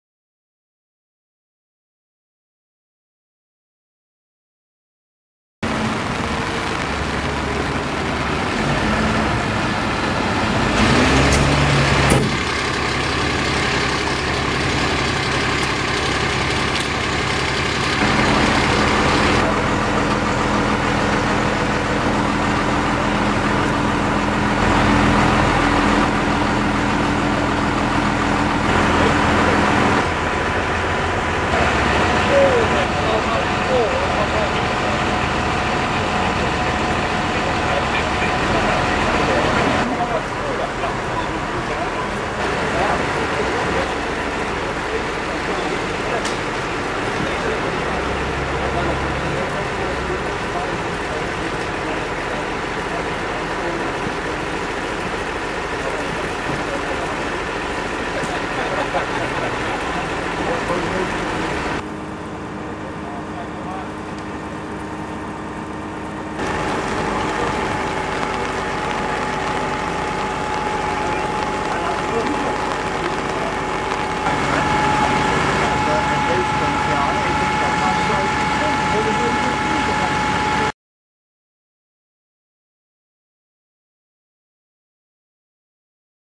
WKK brand aan de Groeneweg
's-Gravenzande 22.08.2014 - De brandweerkorpsen van Maasdijk en De Lier kregen donderdagavond een melding van een agrarische gebouwbrand aan de Groeneweg.